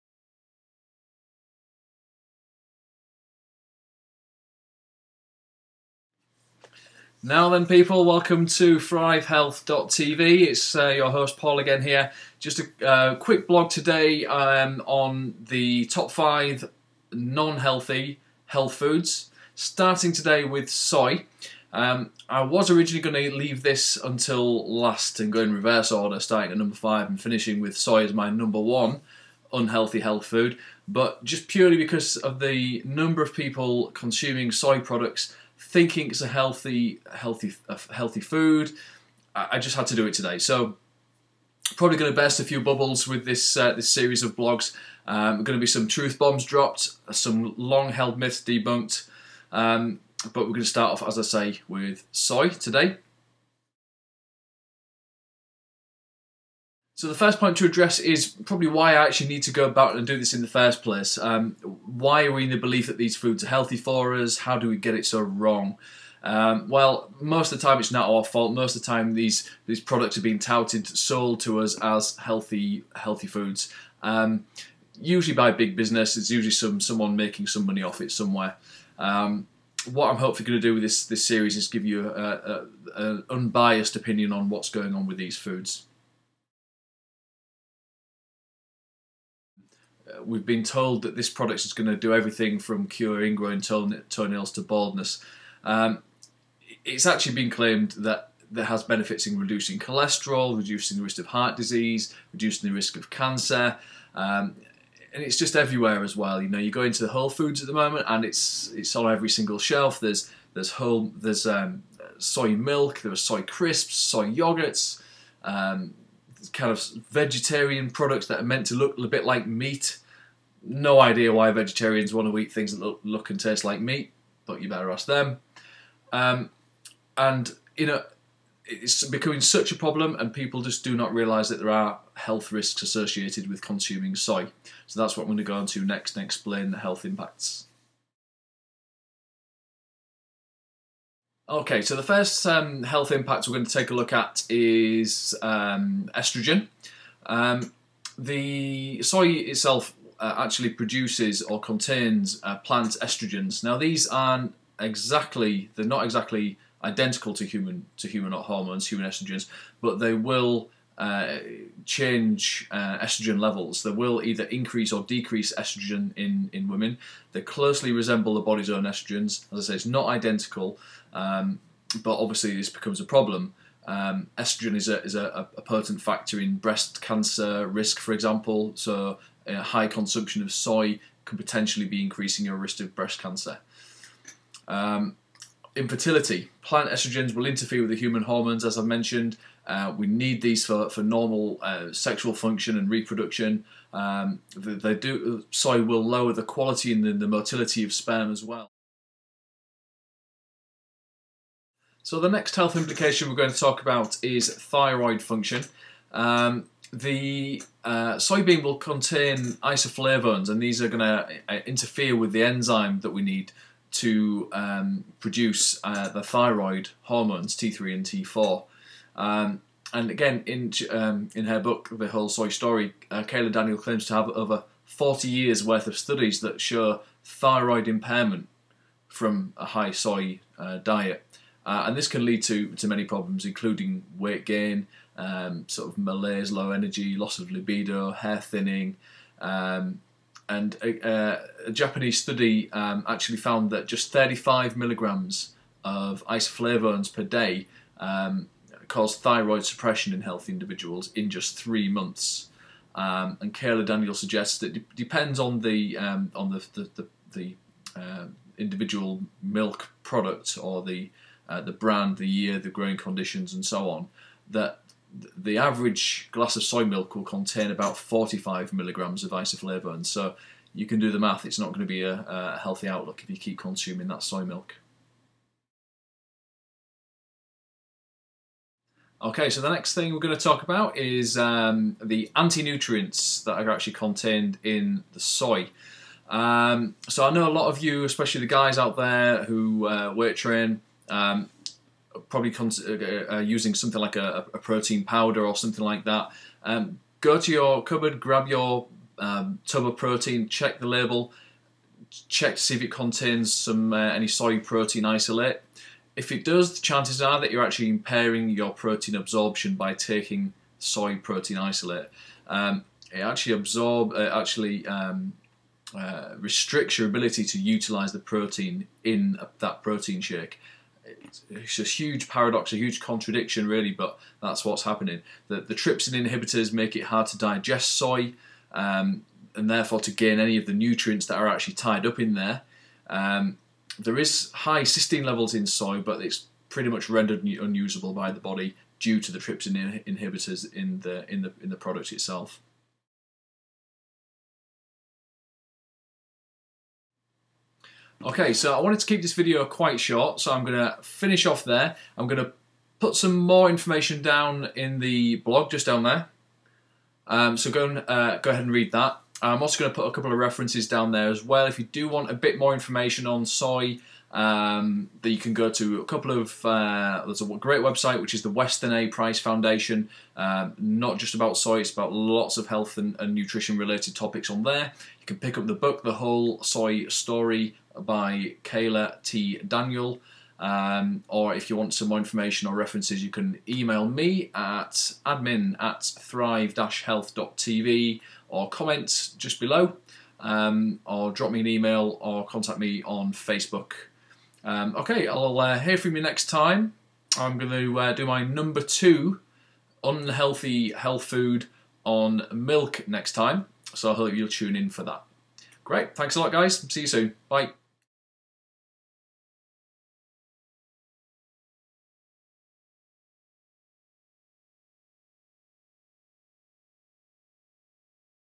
Soy - Number 1 Unhealthy Health Food.m4v
Are you trying to obtain optimal health? Not sure which foods are healthy? This series of videos will let you know which so called health foods are actually far from it.